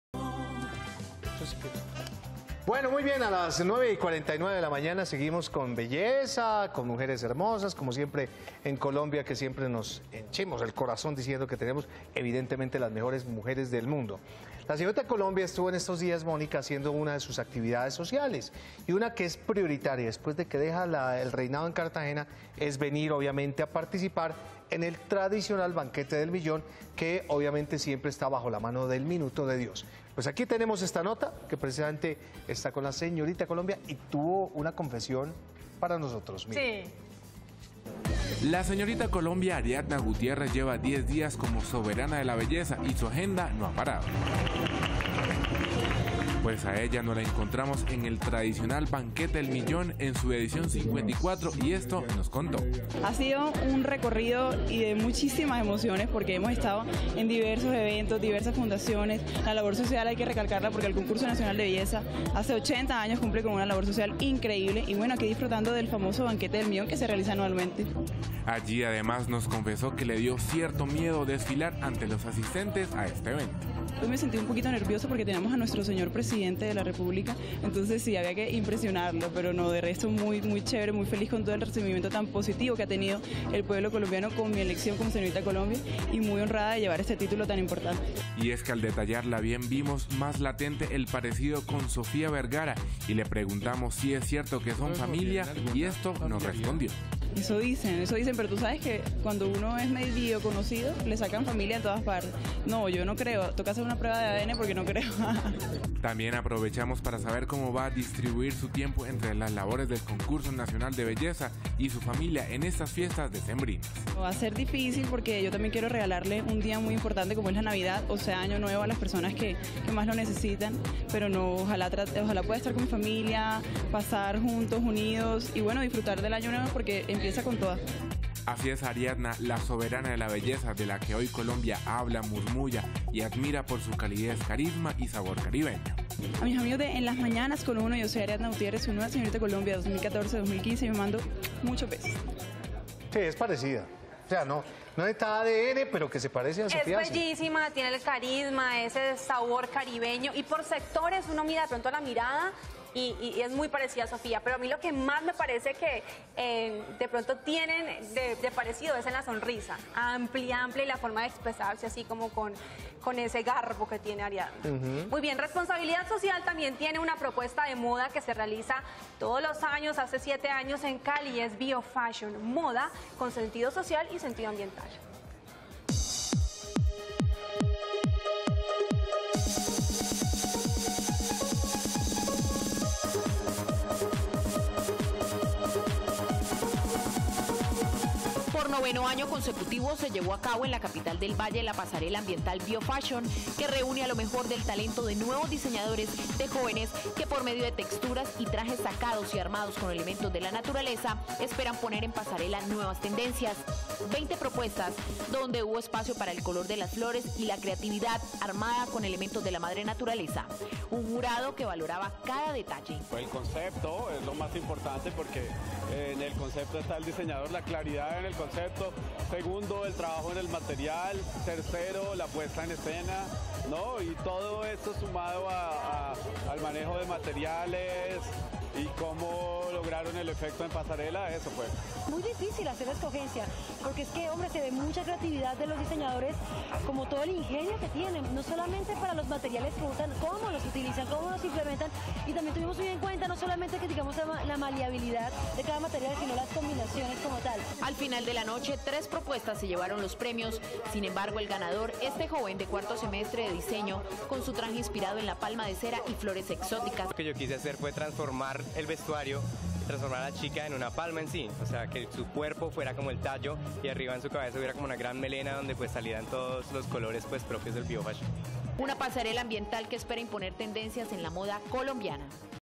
Señorita Colombia engalanó el Banquete del Millón
Nuestro periodista Giancarlo habló con la señorita Colombia Ariadna Gutiérrez sobre su parecido con la actriz Sofía Vergara.